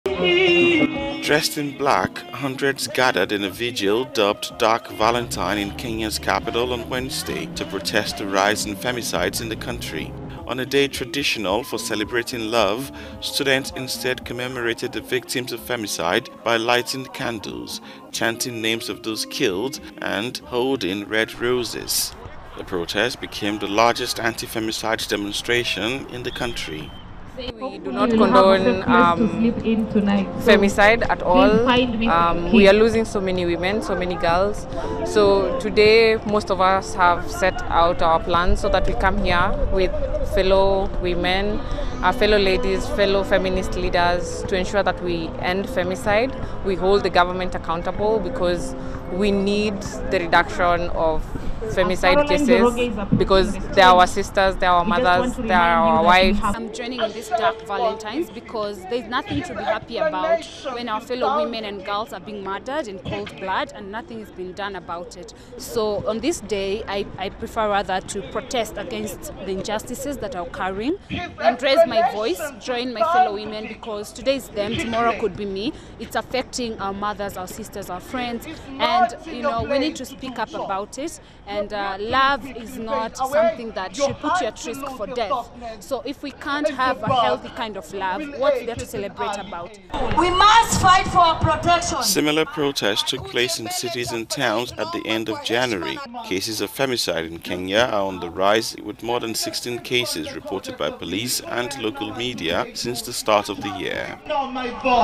Kenya: 'Dark Valentine' in Nairobi as hundreds protest the rise in femicides
"I am joining in this 'Dark Valentine' because there is nothing to be happy about when our fellow women and girls are being murdered in cold blood and nothing has been done about it. On this day, I prefer, rather to, protest against the injustices that are occurring and raise my voice.." Debora Mom